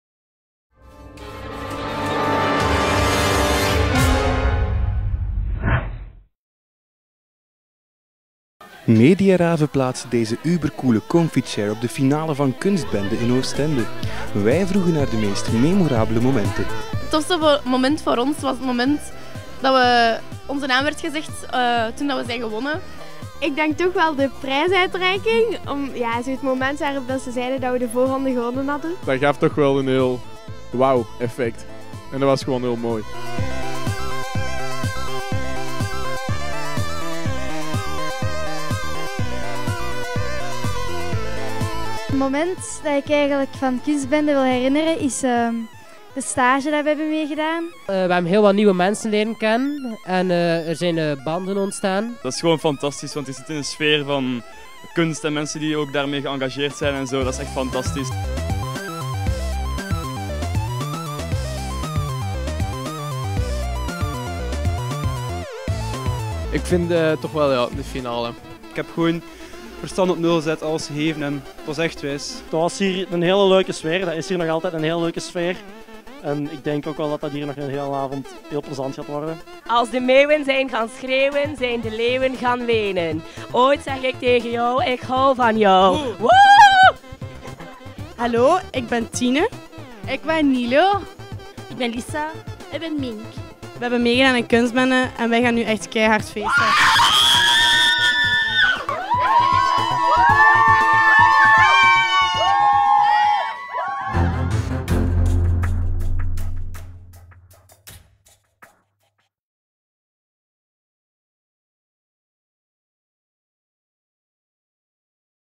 Deelnemers Kunstbende klappen uit de biecht
Onze reporters zetten hun sofa neer, nodigen de finalisten van Kunstbende uit om plaats te nemen en gaan op jacht naar de hoogtepunten van hun Kunstbendeparcours.

Geniet mee van de openheid van dit aanstormende talent.